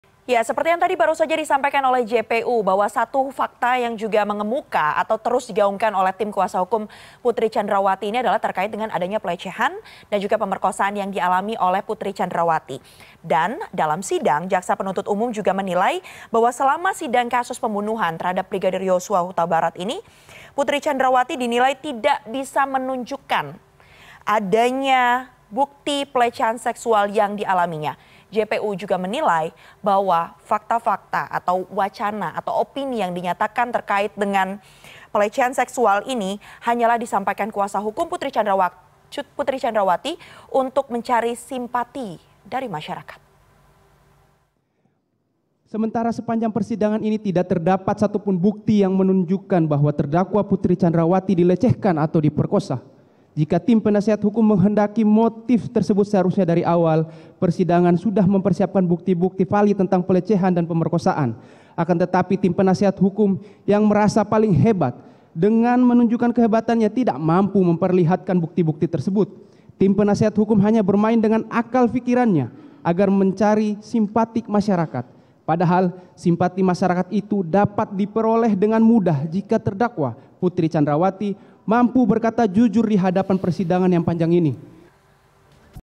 JPU Nilai Kuasa Hukum Tak Bisa Buktikan Pemerkosaan
Sementara itu, Jaksa Penuntut Umum menilai selama sidang kasus pembunuhan Yosua Hutabarat berlangsung, Putri Candrawati tidak bisa menghadirkan bukti yang menunjukan bahwa ia mengalami pelecehan seksual. Mereka menilai tim kuasa hukum hanya mencari simpati masyarakat.